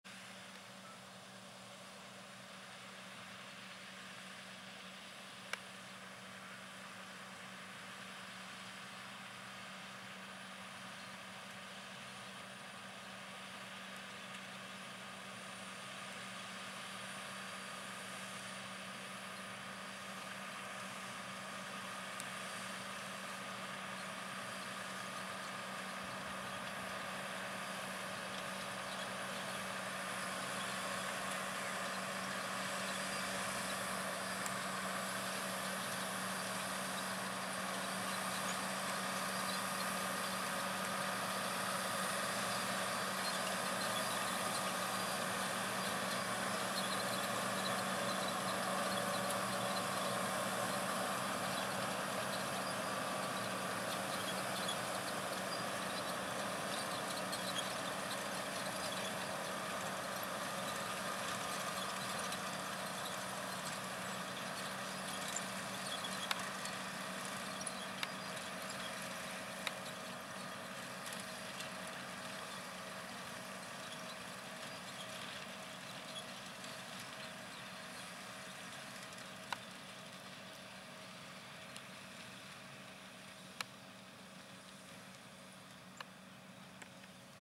Stratheden Pyrethrum Drilling Part 1
Drilling pyrethrum into strip tilled ground at "Stratheden" this afternoon (29th October 2020) with an Amazone ED-Contour precision drill behind a new (a bit over 100 hours on the clock) JCB Fastrac 4220.